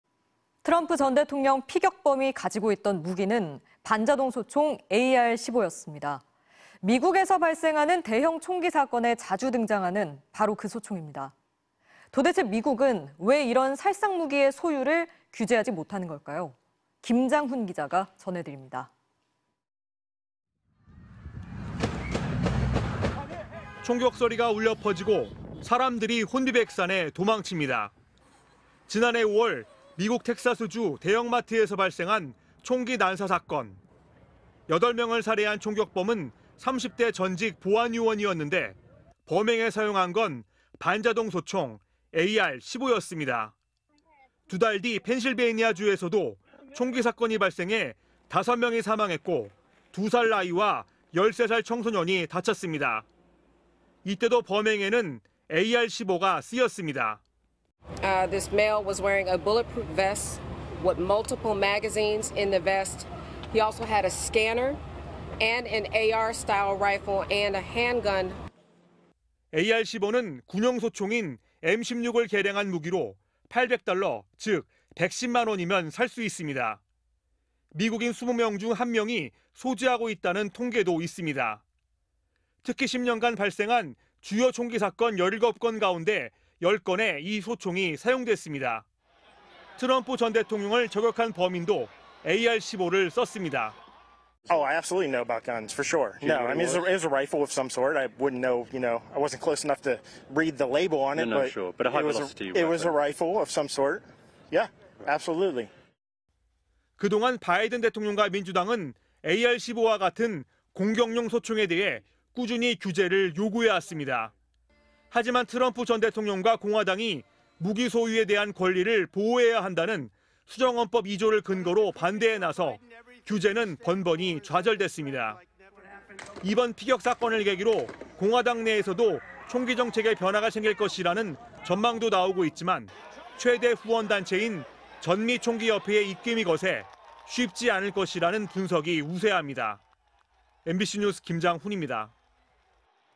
트럼프 쐈던 AR-15‥살상 무기 규제는 왜 번번이 좌절되나? (2024.07.16/뉴스데스크/MBC)
트럼프 전 대통령 피격범이 가지고 있던 무기는 반자동 소총 AR-15였습니다.
미국에서 발생하는 대형 총기 사건에 자주 등장하는 바로 그 소총입니다.
도대체 미국은 왜 이런 살상 무기의 소유를 규제하지 못하는 걸까요?